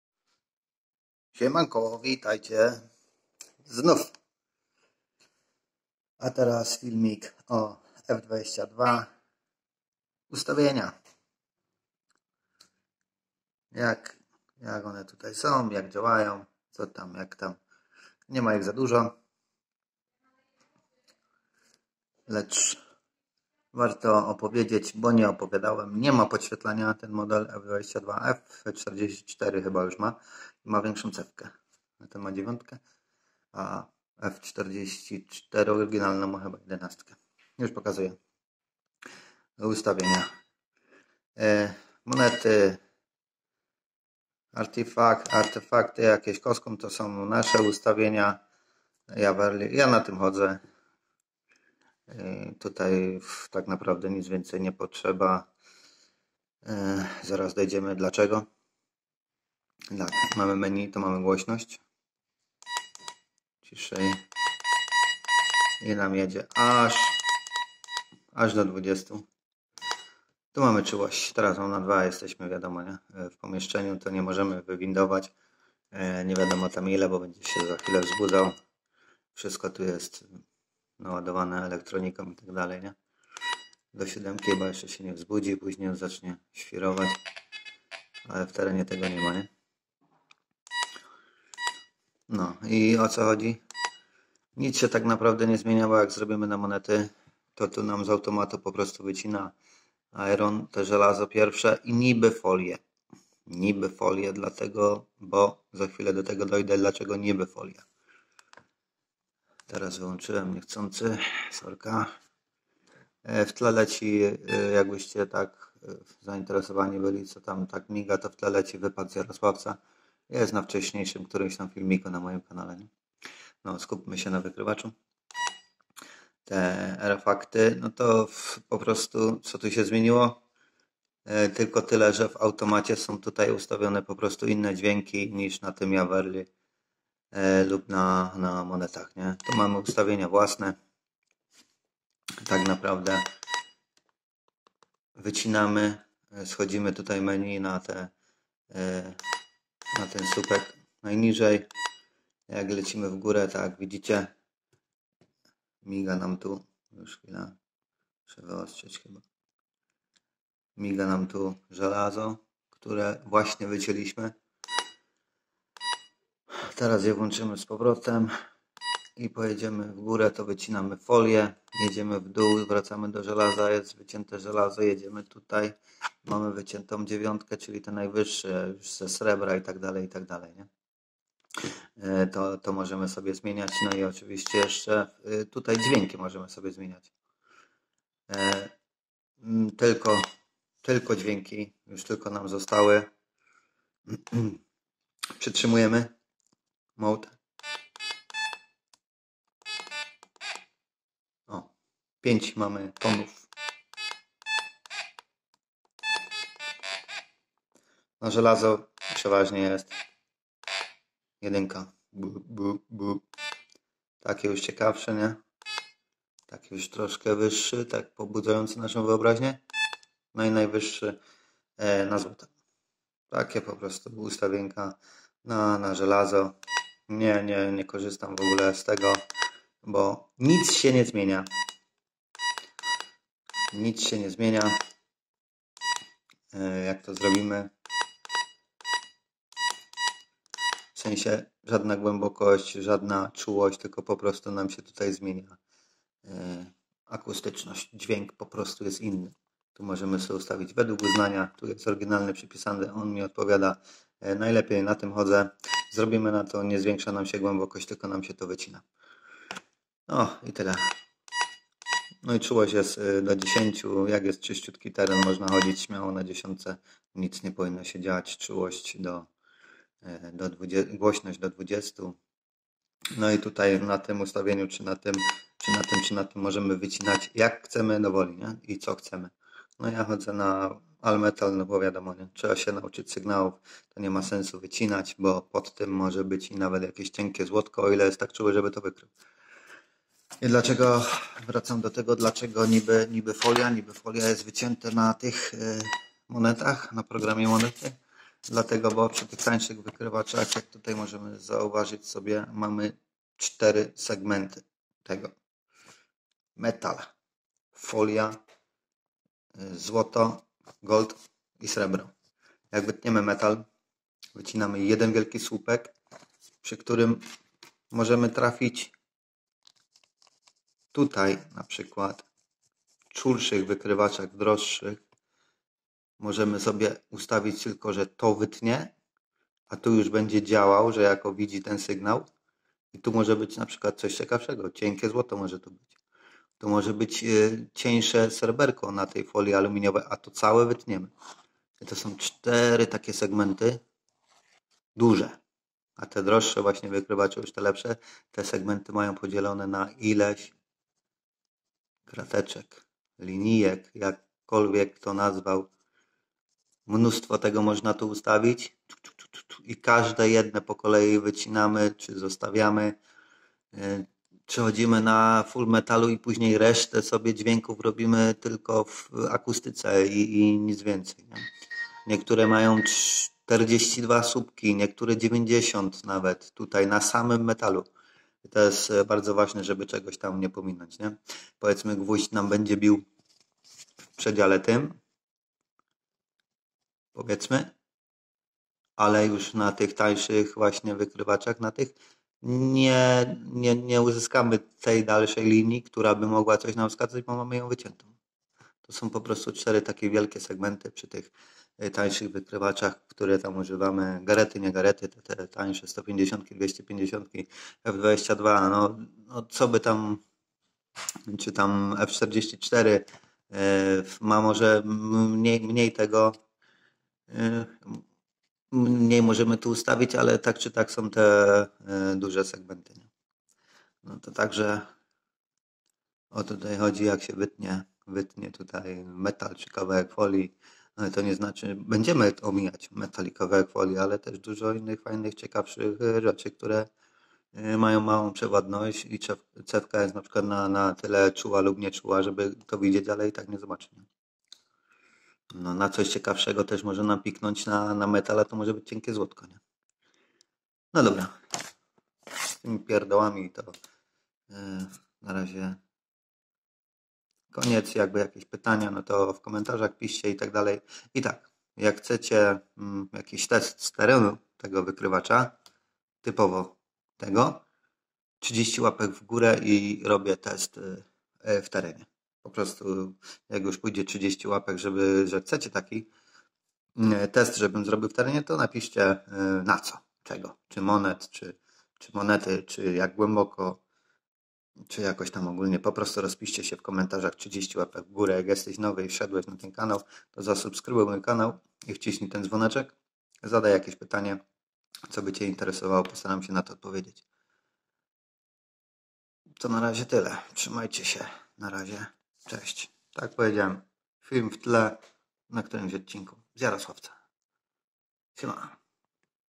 Fisher f22 ustawienia działanie- jaki wykrywacz na początek ?
Jaki wykrywacz na początek ??

Kto bez pozwolenia  poszukuje ukrytych lub porzuconych zabytków, w tym przy użyciu wszelkiego rodzaju urządzeń elektronicznych  podlega grzywnie, karze ograniczenia wolności albo pozbawienia wolności do lat 2, monety współczesne na plażach można poszukiwać, ustawa o rzeczach znalezionych.